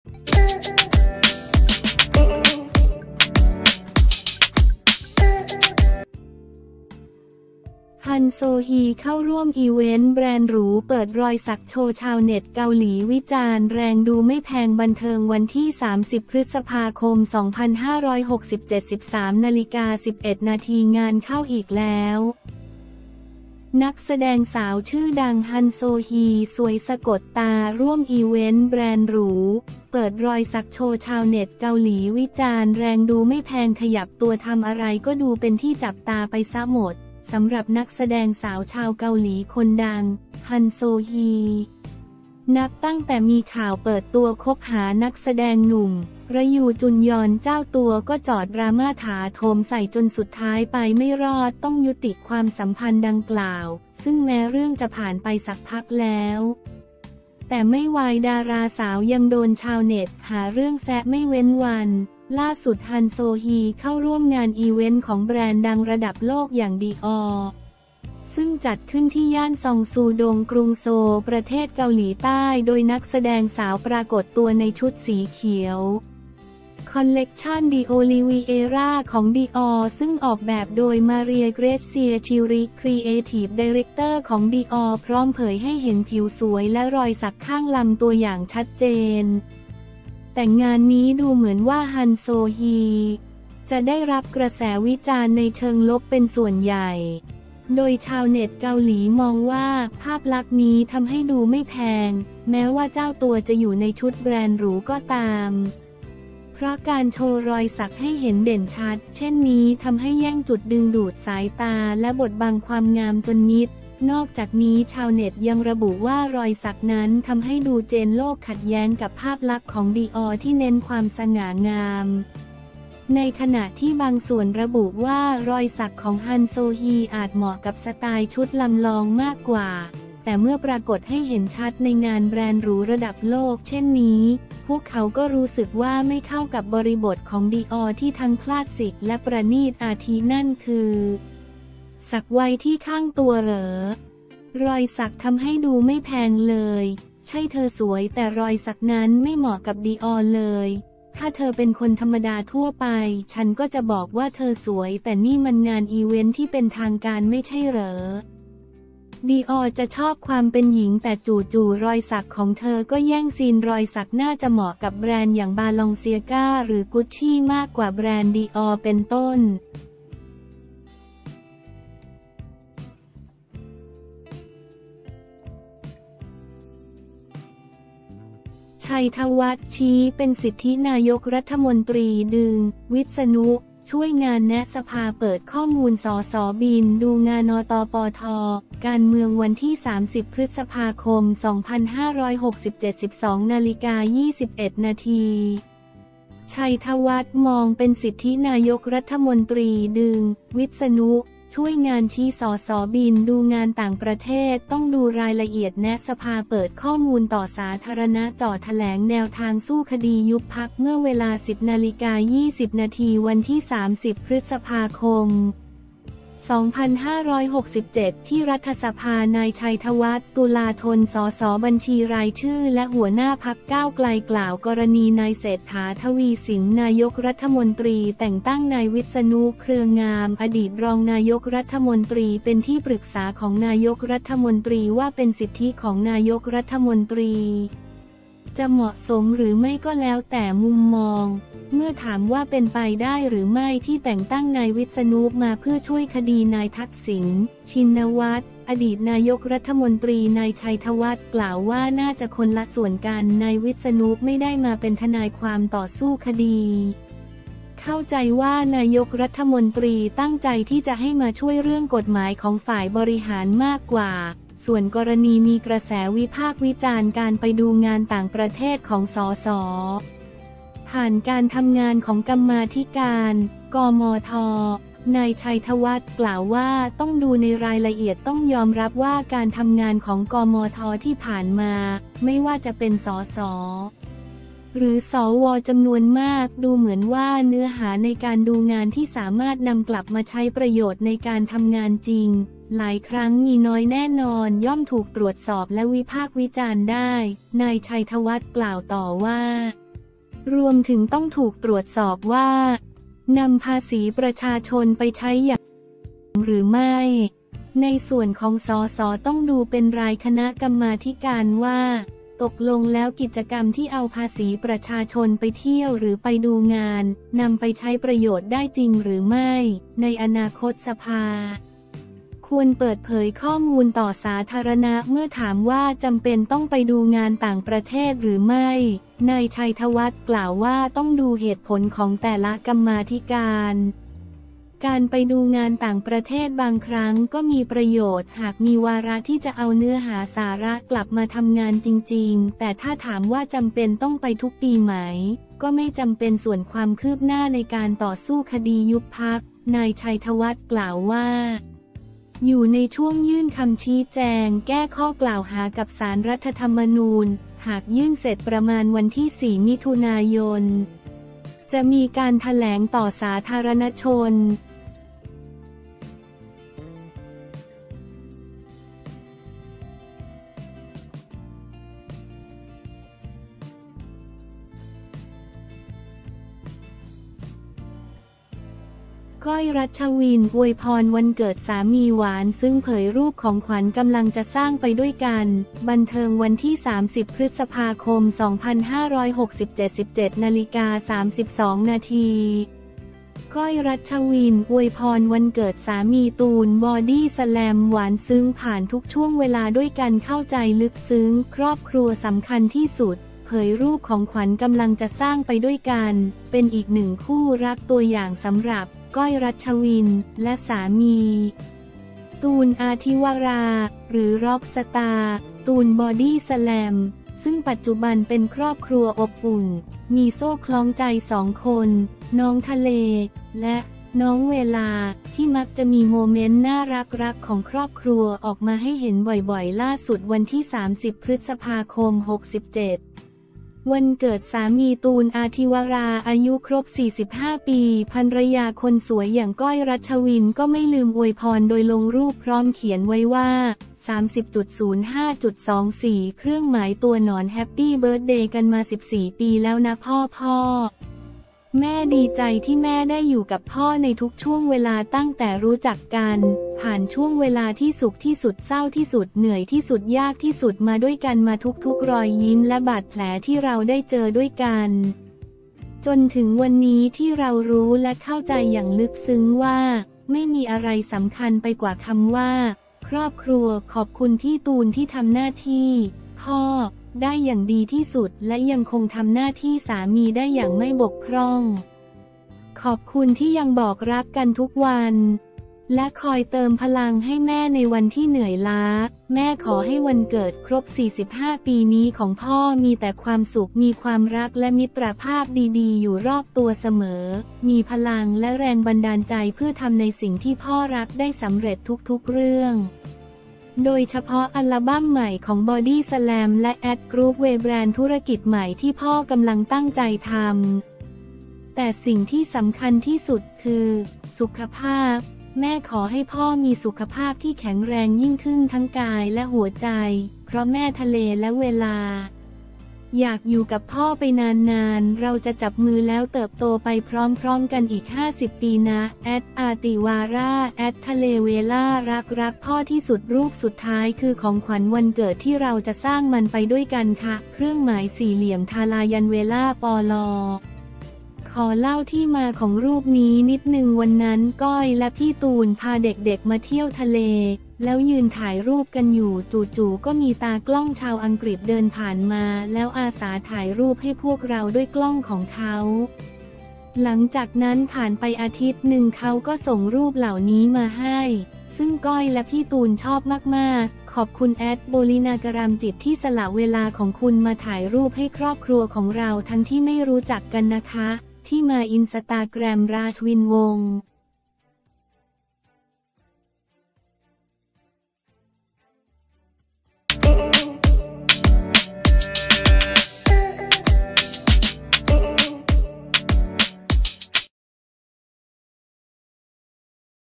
ฮันโซฮี เข้าร่วมอีเวนต์แบรนด์หรู เปิดรอยสักโชว์ ชาวเน็ตเกาหลีวิจารณ์แรง ดูไม่แพง
30 พ.ค. 2567
13:11 น.

งานเข้าอีกแล้ว! นักแสดงสาวชื่อดัง ฮันโซฮี สวยสะกดตาร่วมอีเวนต์แบรนด์หรู เปิดรอยสักโชว์ ชาวเน็ตเกาหลีวิจารณ์แรง ดูไม่แพง

ขยับตัวทำอะไรก็ดูเป็นที่จับตาไปซะหมด สำหรับนักแสดงสาวชาวเกาหลีคนดัง ฮันโซฮี นับตั้งแต่มีข่าวเปิดตัวคบหานักแสดงหนุ่ม รยูจุนยอล เจ้าตัวก็เจอดราม่าถาโถมใส่ จนสุดท้ายไปไม่รอดต้องยุติความสัมพันธ์ดังกล่าว ซึ่งแม้เรื่องจะผ่านไปสักพักแล้ว แต่ไม่วายดาราสาวยังโดนชาวเน็ตหาเรื่องแซะไม่เว้นวัน
ล่าสุด ฮันโซฮี เข้าร่วมงานอีเวนต์ของแบรนด์ดังระดับโลกอย่าง Dior ซึ่งจัดขึ้นที่ย่านซองซูดง กรุงโซล ประเทศเกาหลีใต้ โดยนักแสดงสาวปรากฏตัวในชุดสีเขียว คอลเลกชั่น Dioliviera ของ Dior ซึ่งออกแบบโดย Maria Grazia Chiuri ครีเอทีฟไดเร็กเตอร์ของ Dior พร้อมเผยให้เห็นผิวสวยและรอยสักข้างลำตัวอย่างชัดเจน

แต่งานนี้ดูเหมือนว่า ฮันโซฮี จะได้รับกระแสวิจารณ์ในเชิงลบเป็นส่วนใหญ่ โดยชาวเน็ตเกาหลีมองว่าภาพลักษณ์นี้ทำให้ดูไม่แพง แม้ว่าเจ้าตัวจะอยู่ในชุดแบรนด์หรูก็ตาม เพราะการโชว์รอยสักให้เห็นเด่นชัดเช่นนี้ทำให้แย่งจุดดึงดูดสายตาและบดบังความงามจนมิด
นอกจากนี้ชาวเน็ตยังระบุว่า รอยสักนั้นทำให้ดูเจนโลกขัดแย้งกับภาพลักษณ์ของ Dior ที่เน้นความสง่างาม ในขณะที่บางส่วนระบุว่ารอยสักของฮันโซฮีอาจเหมาะกับสไตล์ชุดลำลองมากกว่า แต่เมื่อปรากฏให้เห็นชัดในงานแบรนด์หรูระดับโลกเช่นนี้ พวกเขาก็รู้สึกว่าไม่เข้ากับบริบทของ Dior ที่ทั้งคลาสสิกและประณีต
อาทิ นั่นคือ สักไว้ที่ข้างตัวเหรอ?, รอยสักทำให้ดูไม่แพงเลย, ใช่ เธอสวย แต่รอยสักนั้นไม่เหมาะกับดิออร์เลย, ถ้าเธอเป็นคนธรรมดาทั่วไป ฉันก็จะบอกว่าเธอสวย แต่นี่มันงานอีเวนต์ที่เป็นทางการไม่ใช่เหรอ? Dior จะชอบความเป็นหญิง แต่จู่ๆ รอยสักของเธอก็แย่งซีน รอยสักน่าจ